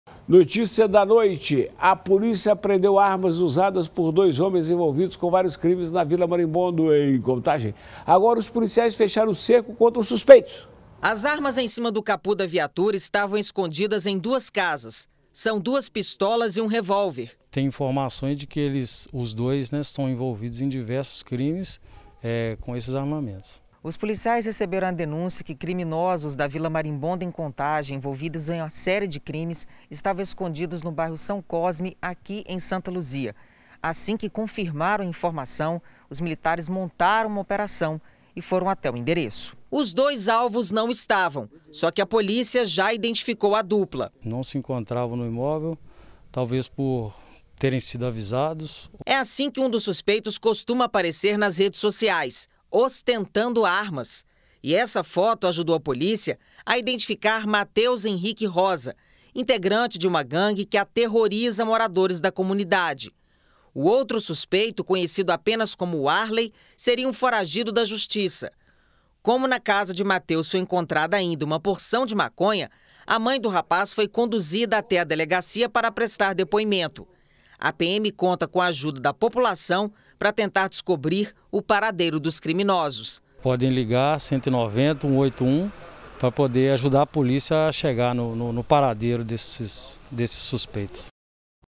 Polícia apreende armas usadas por suspeitos em Contagem, na Grande BH
A polícia apreendeu armas usadas por dois homens envolvidos com vários crimes na Vila Marimbondo, em Contagem, na Grande BH. Agora, os policiais fecharam o cerco contra os suspeitos.